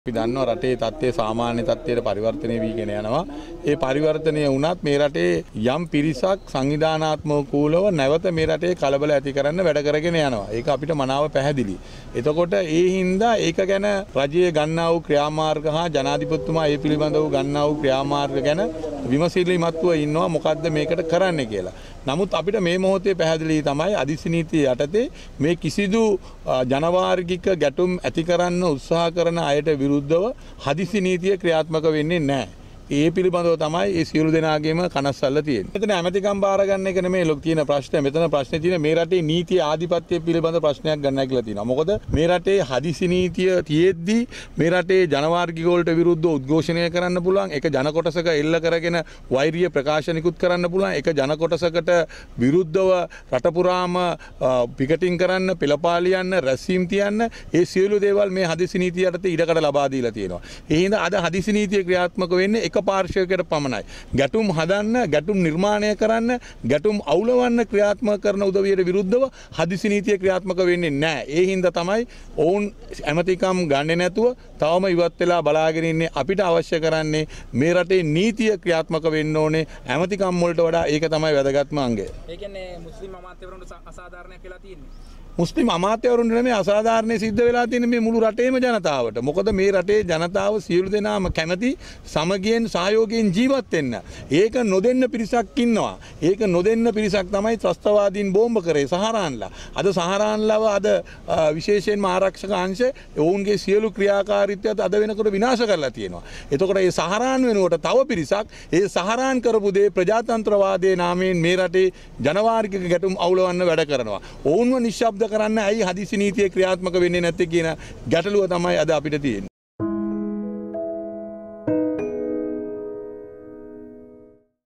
Emergency Regulations are partial | LNW
හදිසි නීතිය ක්‍රියාත්මක වන්නේ එක් කොටසකට පමණයි